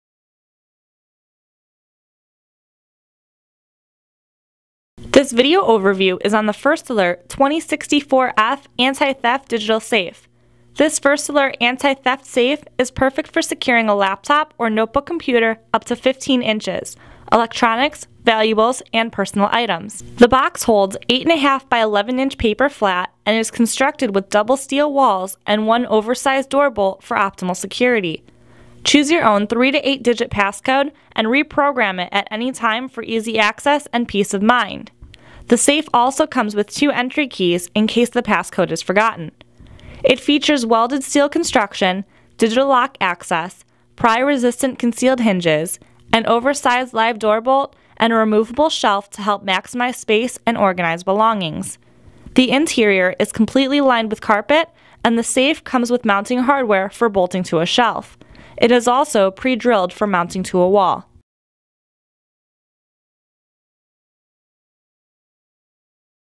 First Alert 2064F Anti-Theft Digital Safe
This First Alert anti-theft safe is perfect for securing a laptop or notebook computer (up to 15 inches), electronics, valuables and personal items. The box holds 8-1/2 by 11-inch paper flat, and is constructed with double steel walls and one oversized door bolt for optimal security. Choose your own 3 to 8 digit passcode and reprogram it any time for easy access and peace of mind. It features welded steel construction, digital lock access, pry-resistant concealed hinges, an oversized live door bolt, and a removable shelf to help maximize space and organize belongings. The interior is completely lined with carpet and the safe comes with mounting hardware for bolting to a shelf. It is also pre-drilled for mounting to a wall.